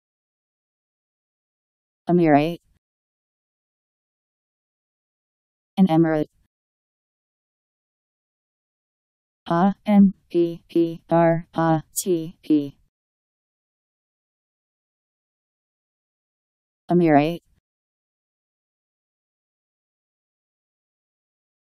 Ameerate Meaning
Video shows what ameerate means. An emirate.  Ameerate Meaning. How to pronounce, definition audio dictionary. How to say ameerate. Powered by MaryTTS, Wiktionary